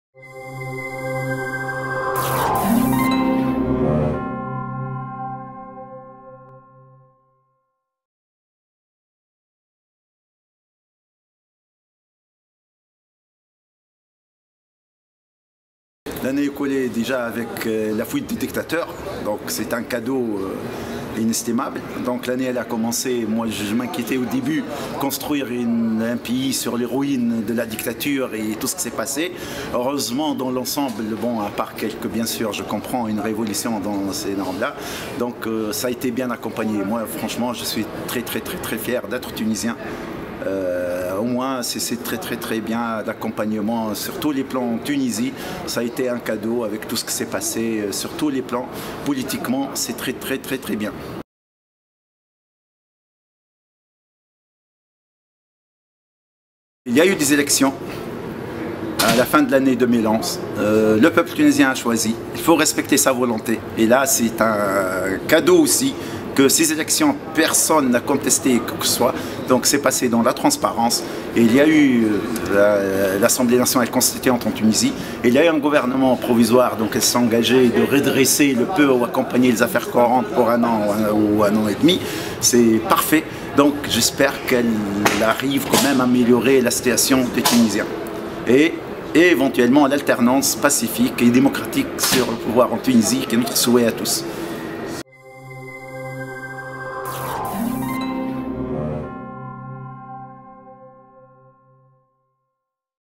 Interview Politique Tarek Toukabri
Un an après la révolution tunisienne, c'est l'heure du bilan et des perspectives.